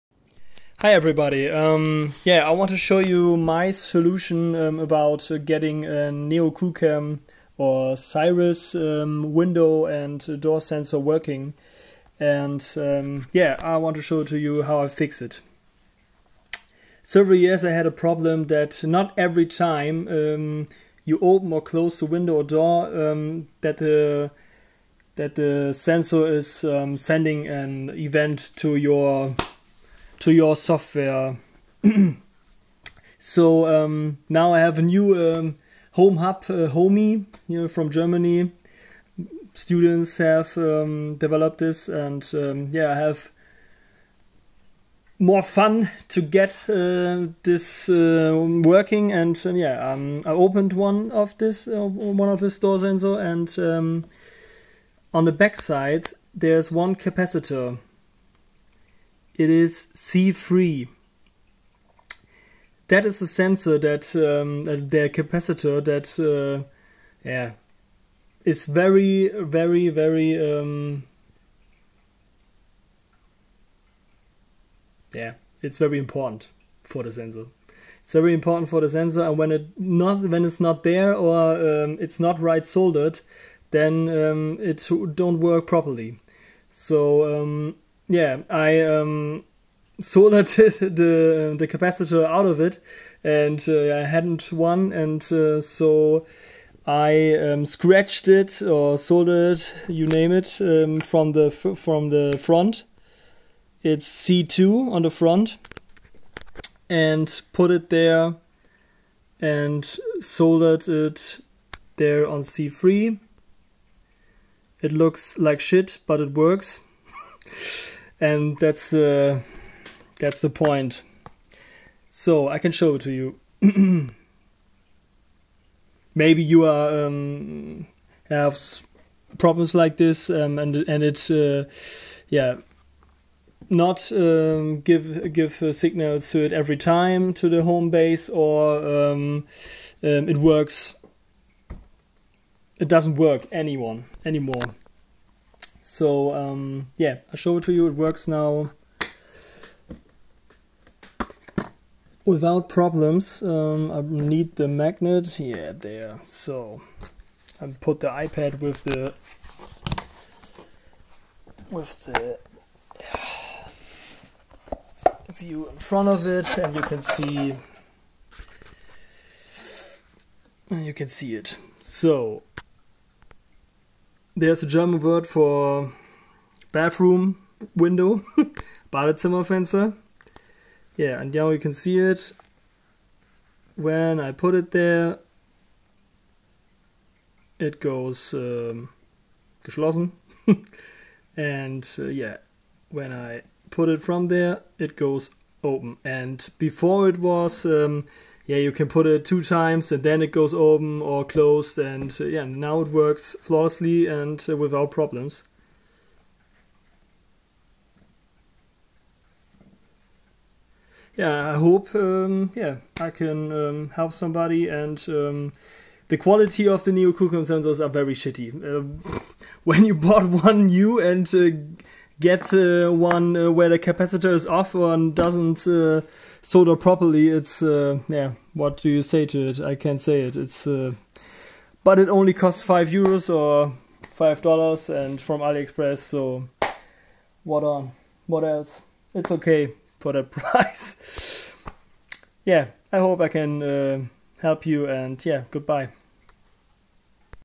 Repairing Neo Coolcam / Cyrus Window / Door Sensor | Not working or not every time
If you have a broken NeoCoolcam or Cyrus Window / DoorSensor and it doesn't respond every time or doesn't work it all, here is the solution. You need an Soldering Iron and patience.

Replace or solder the C3  Capacitor on the backside. You can replace it with the C2 from the front of the sensor. Now it works flawlessly.

I hope I can help you a little bit with this video.